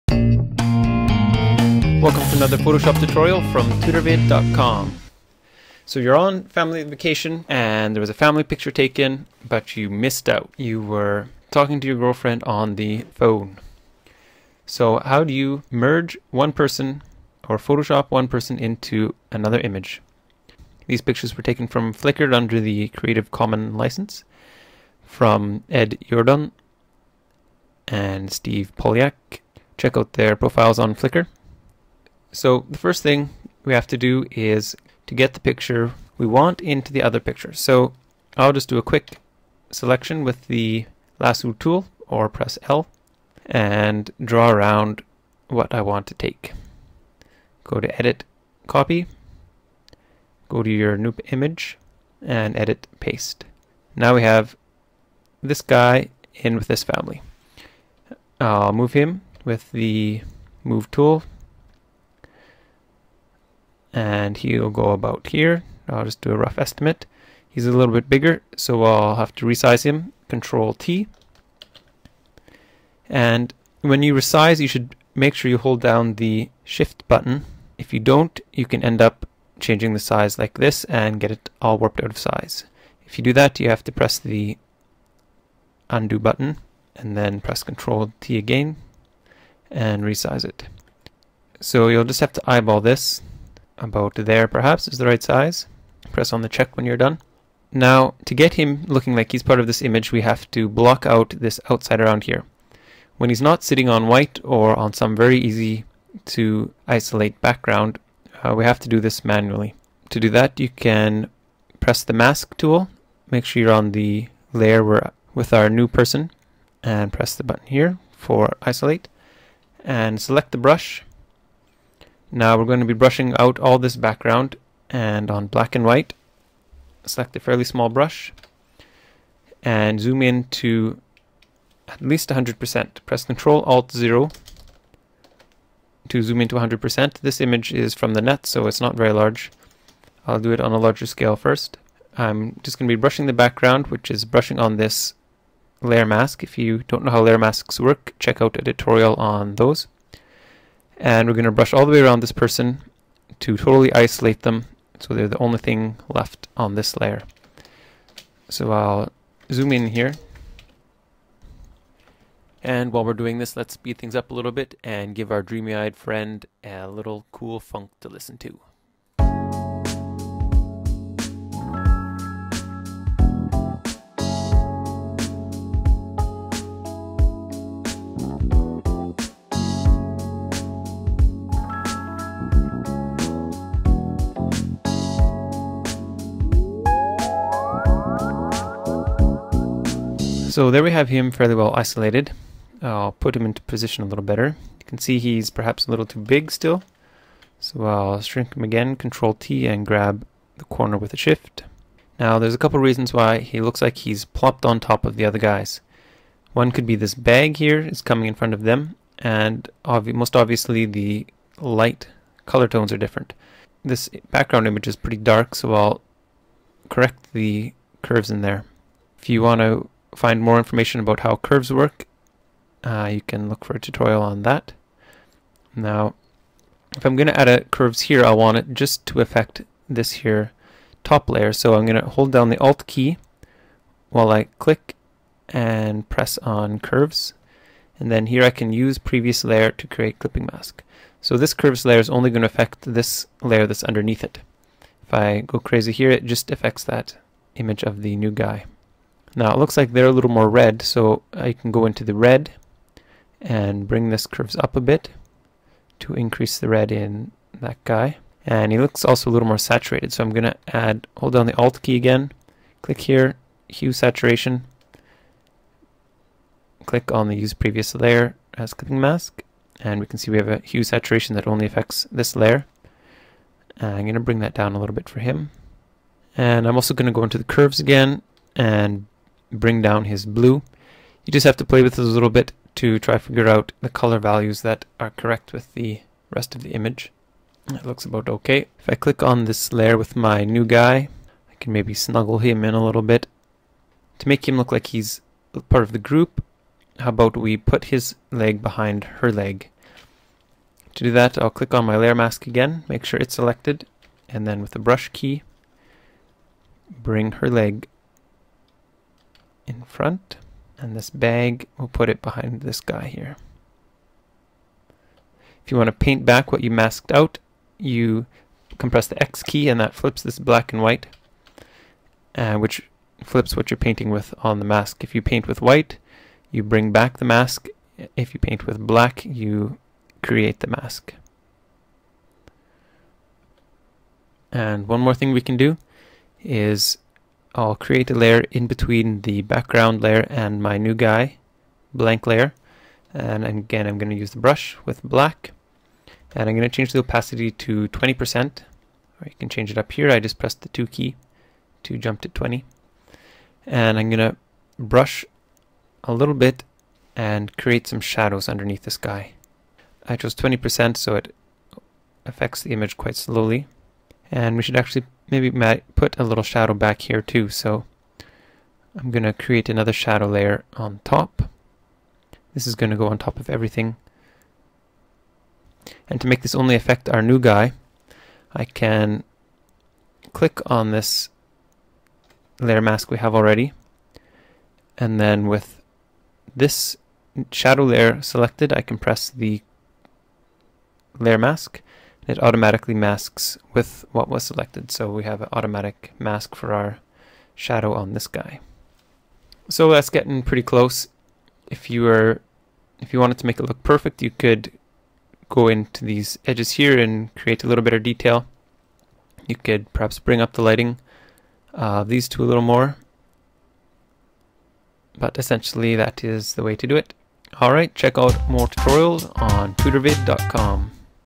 Photoshop Tutorial: How to Photoshop a Person Into a Picture [In-Depth]
An in-depth tutorial with step-by-step directions on using Photoshop to edit a person into a photo.

Want to know how to do something in Photoshop? Request a tutorial in the comments or post here